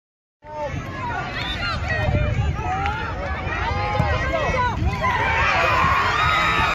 Soccer and Dad visit 20141122(9)
Joe's AYSO soccer tournament and Dad visit.